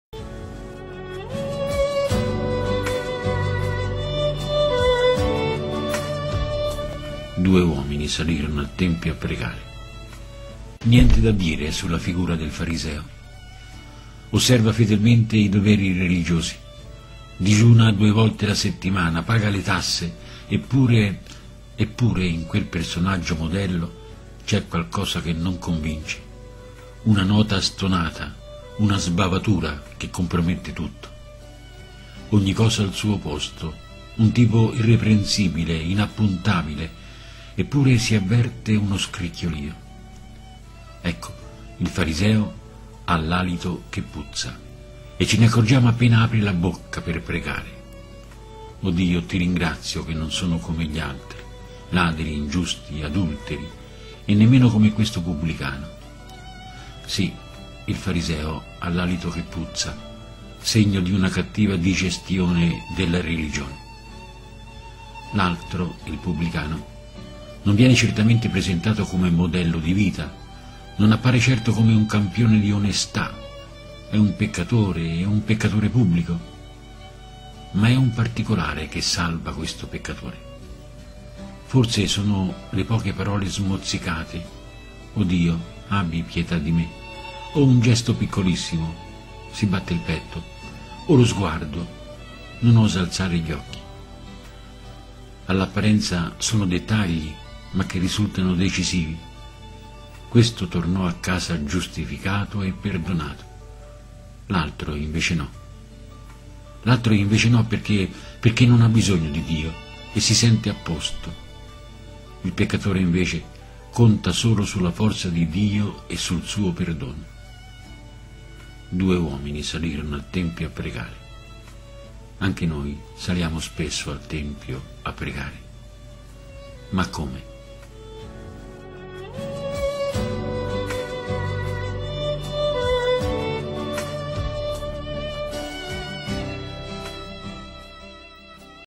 Pensiero della settimana (1 settimana settembre)
Due uomini salirono al tempio a pregare.
Niente da dire sulla figura del fariseo: osserva fedelmente i doveri religiosi, digiuna due volte alla settimana, paga le tasse, eppure ...
Eppure in quel personaggio modello c'è qualcosa che non convince, una nota stonata, una sbavatura che compromette tutto. Ogni cosa al suo posto, un tipo irreprensibile, inappuntabile, eppure si avverte uno scricchiolio. Ecco, il fariseo ha l'alito che puzza e ce ne accorgiamo appena apre la bocca per pregare: O Dio, ti ringrazio che non sono come gli altri, ladri, ingiusti, adulteri. E nemmeno come questo pubblicano.
Sì, il fariseo ha l'alito che puzza, segno di una cattiva digestione della religione. L'altro, il pubblicano, non viene certamente presentato come modello di vita, non appare certo come un campione di onestà, è un peccatore e un peccatore pubblico, ma è un particolare che salva questo peccatore. Forse sono le poche parole smozzicate: O Dio, abbi pietà di me. O un gesto piccolissimo: si batte il petto; o lo sguardo: non osa alzare gli occhi.
All'apparenza sono dettagli, ma che risultano decisivi. Questo tornò a casa giustificato e perdonato, l'altro invece no. L'altro invece no perché non ha bisogno di Dio e si sente a posto. Il peccatore invece conta solo sulla forza di Dio e sul suo perdono.
Due uomini salirono al tempio a pregare. Anche noi saliamo spesso al tempio a pregare, ma come?